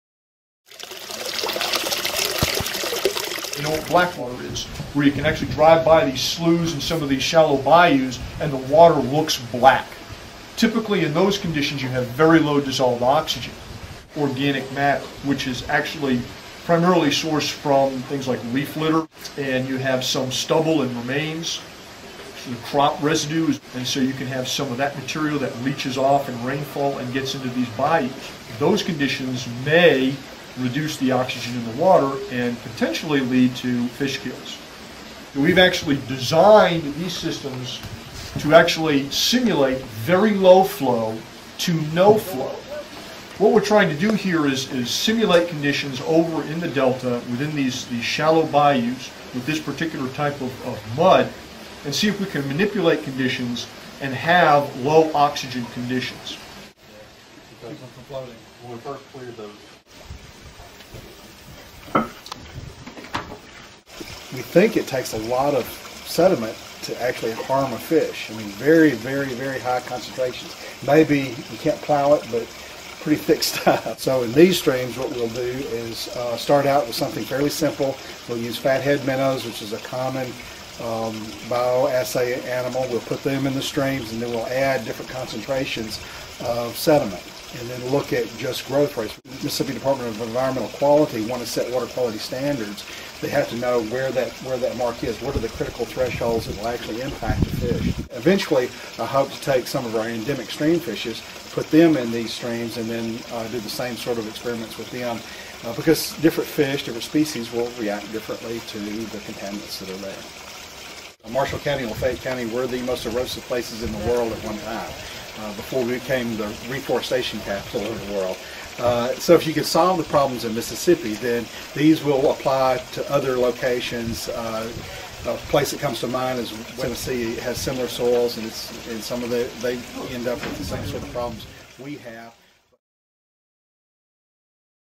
Artificial Streams - Like
UM researchers Scott Knight and Rich Lizotte have started a new research project at the Ole Miss Field Station that involves building artificial streams to simulate Delta wetlands. The research is examining the oxygen content of the water and soil. Learn more about the project by watching the video. Video by Tobie Baker.